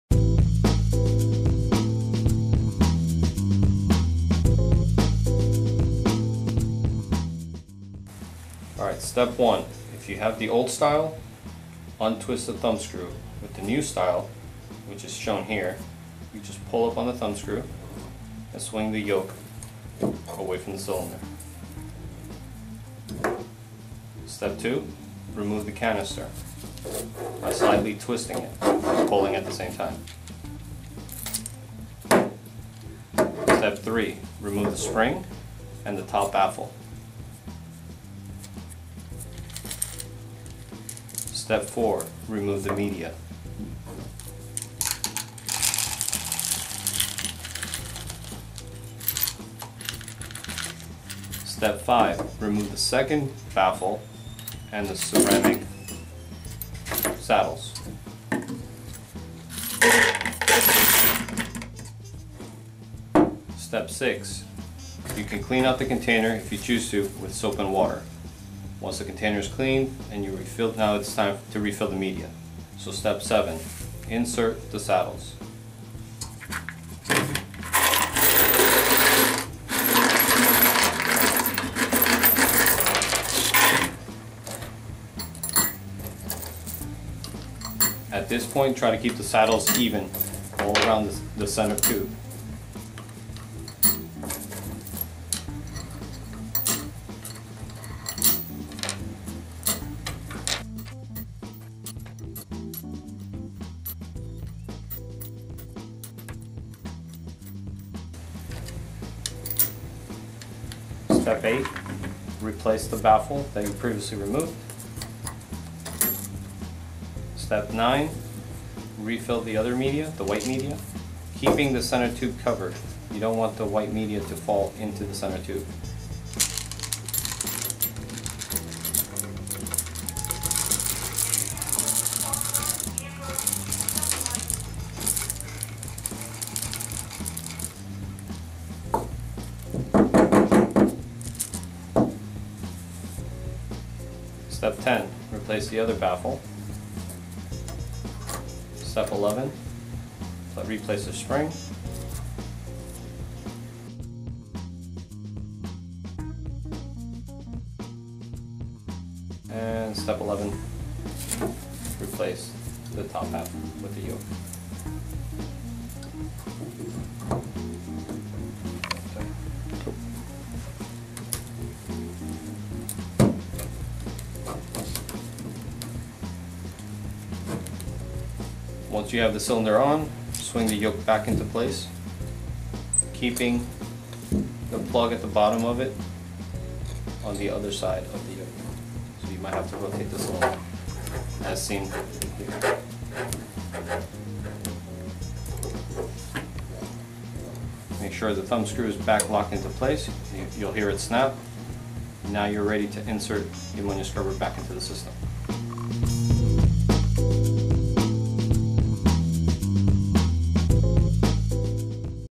Perma Pure AS Series Ammonia Scrubber Media Replacement
This video shows you how to replace the media in your AS Series Ammonia Scrubber. The Ammonia Scrubber can be purchased separately or can be found in our GASS 2040 and MiniGASS Sample Conditioning Systems.